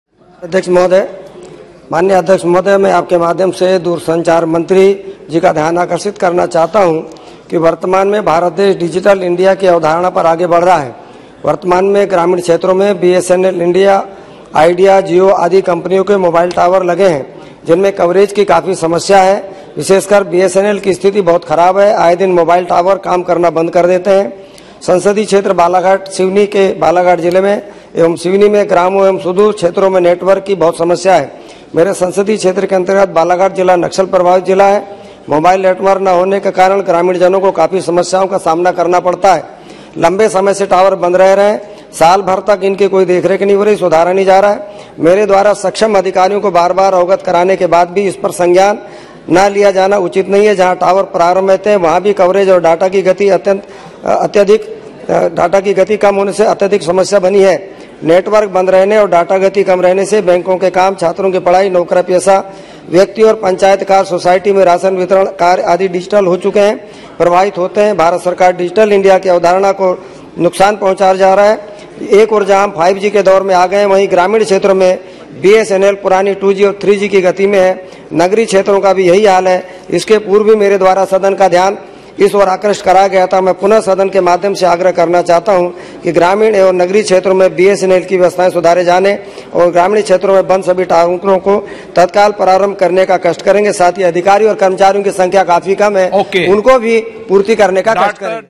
Dr. Dhal Singh Bisen on matters raised with the permission of the chair in Lok Sabha: 07.12.2022
Dr. Dhal Singh Bisen on matters raised with the permission of the chair in Lok Sabha.